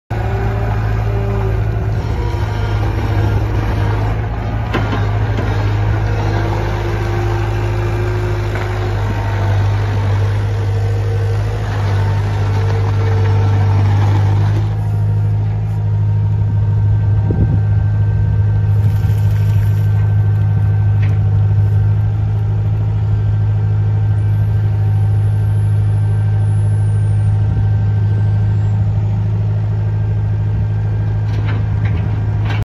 1982 BOBCAT 743 For Sale
VIDEOS AVAILABLE!!  MACHINE RUNS AND OPERATES GOOD. BUCKET IN GOOD CONDITION.